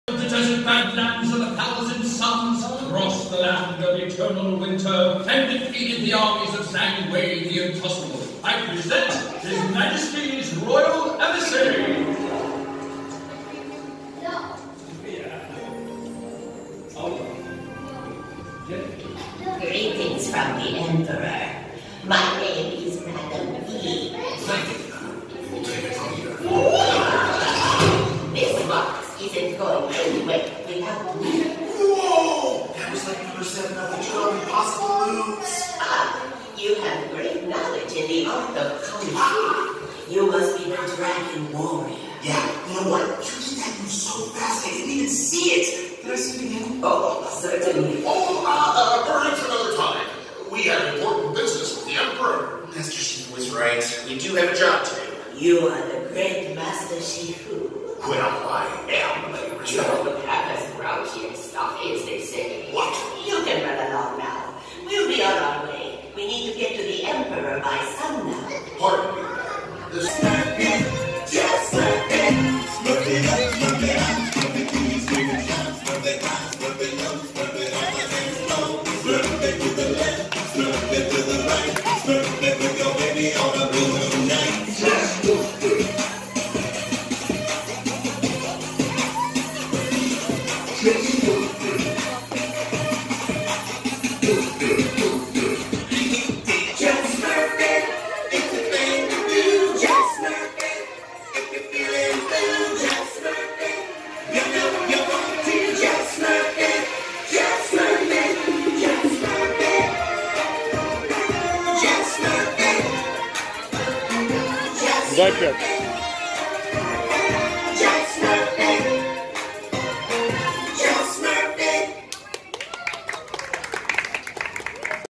😍😍Best Puppet Theater Show | Kids Dance Show | Kids Channel Adriana
Enjoy the best puppet theater show for kids. It's an exciting kids' show about cartoon heroes.  After the puppet show, there is a dancing show for kids! This is amazing. Kids channel Adriana shows the best kids videos on fun, adventures, and kids day out!